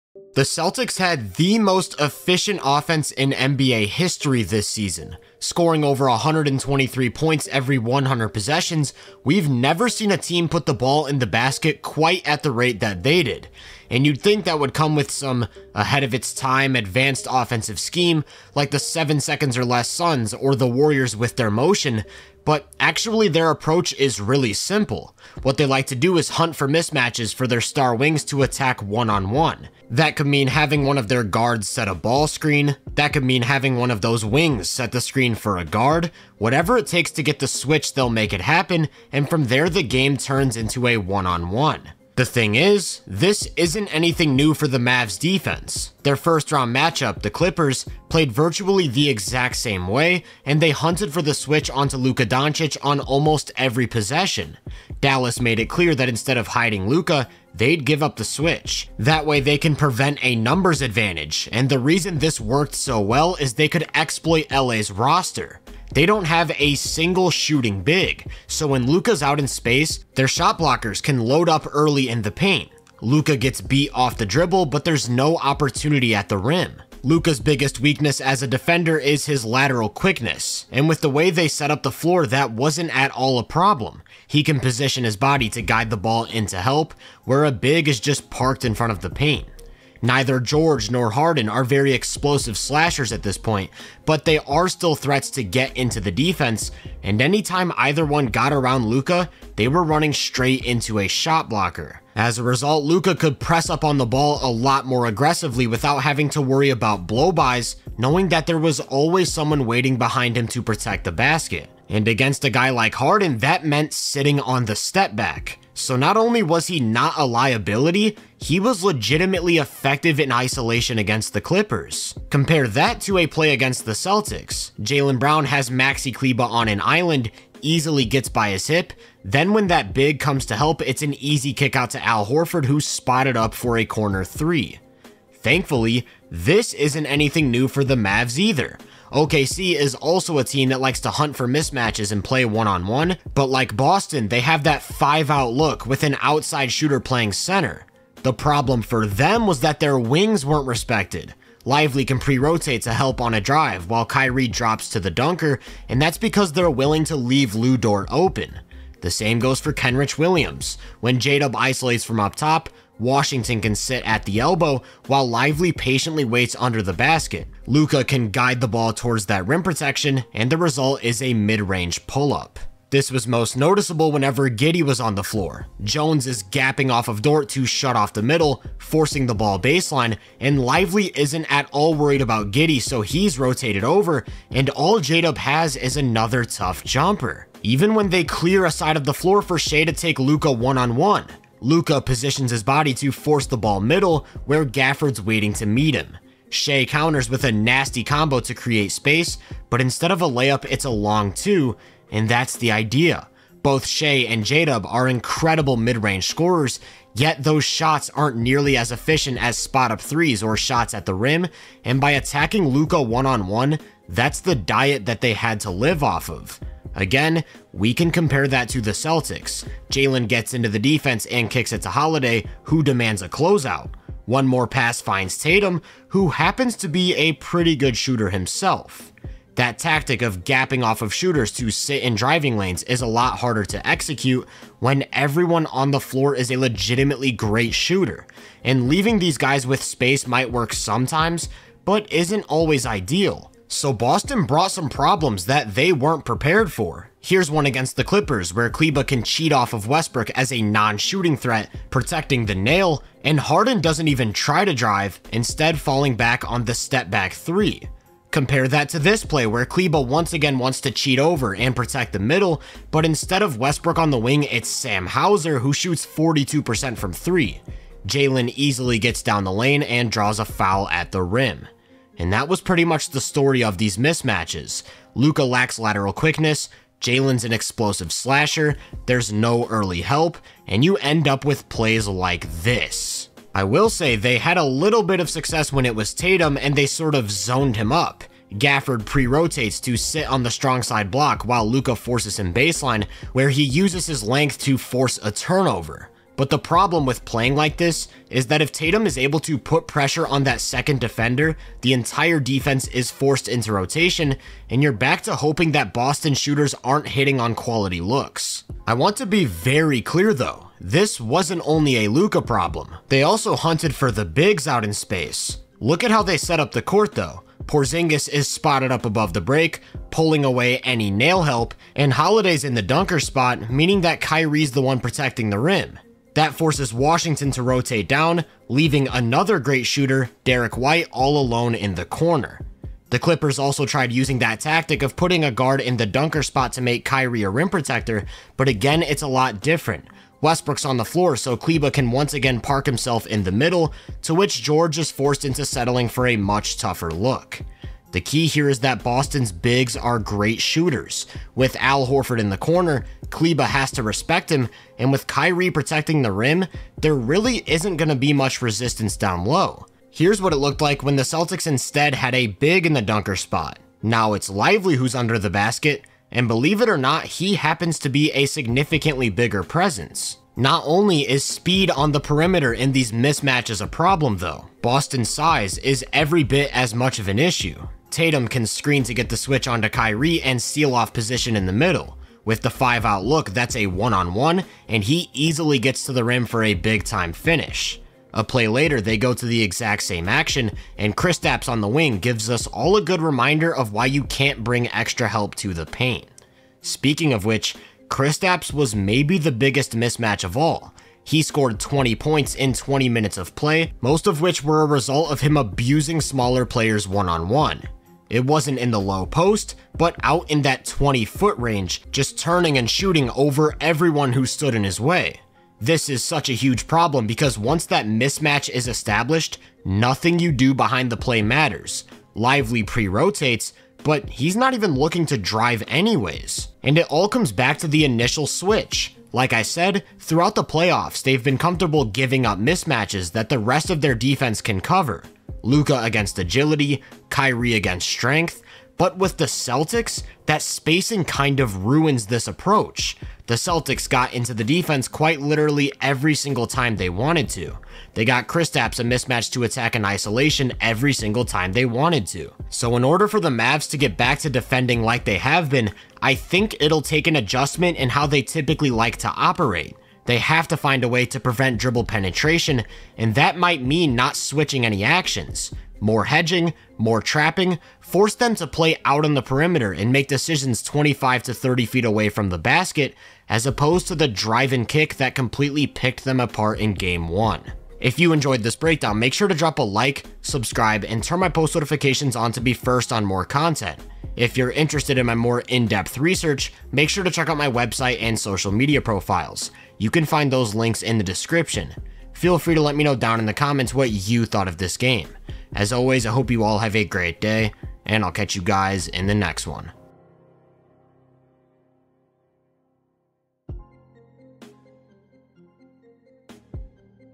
This Doesn't Work Against The Celtics...
The Celtics' style of offense isn't anything the Mavs haven't seen, but the personnel makes it impossibly tough to defend.